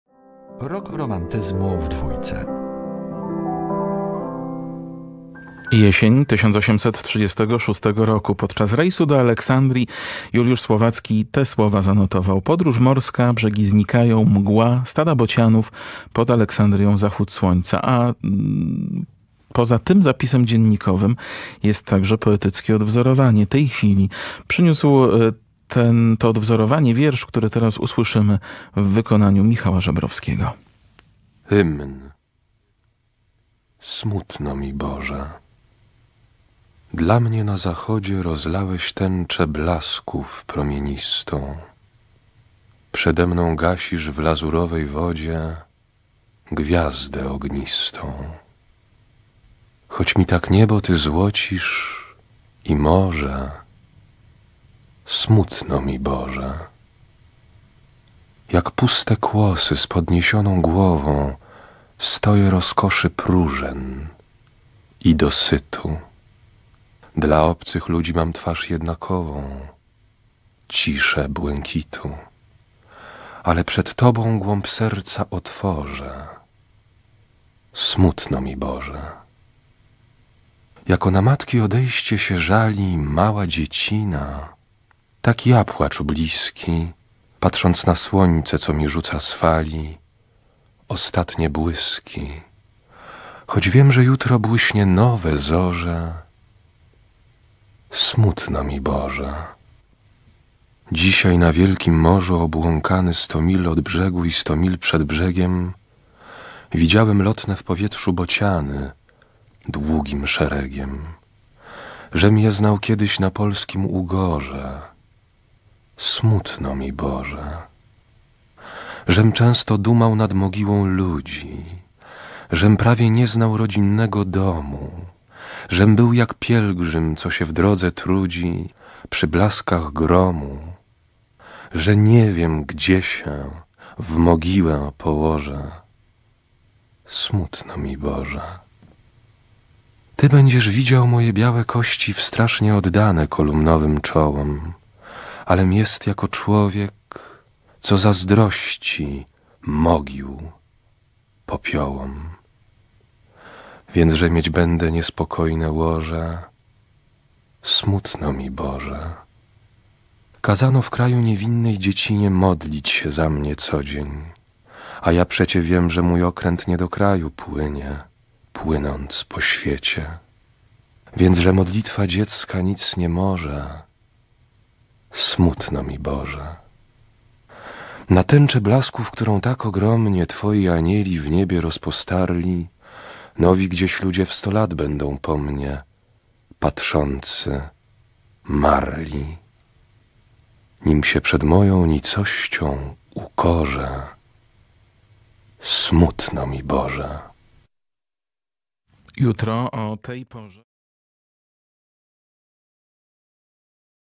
Juliusz Słowacki Hymn
Czyta Michał Żebrowski